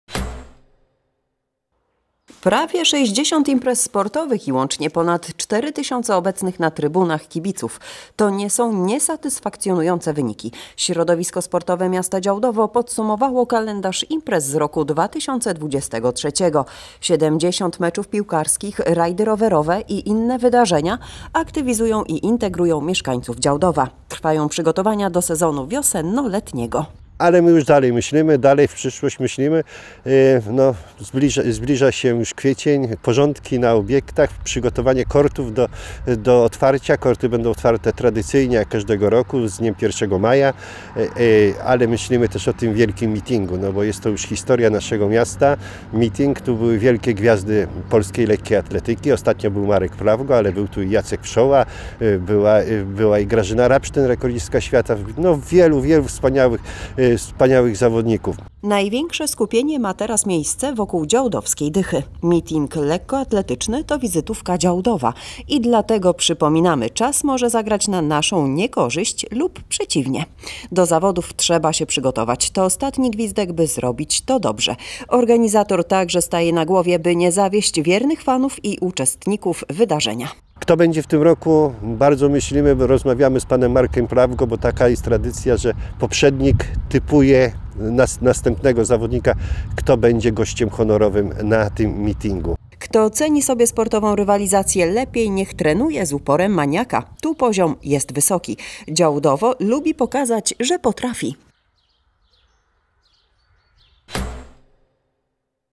DZIAŁDOWO SPORTEM STOI
Działdowo zwalniać nie zamierza. Poprzedni rok pokazał jak dużo udało się zrealizować zadań sportowych. Impreza goniła imprezę. I to dosłownie. MOSiR w Działdowie jest m. in. w trakcie przygotowań do Mityngu Lekkoatletycznego.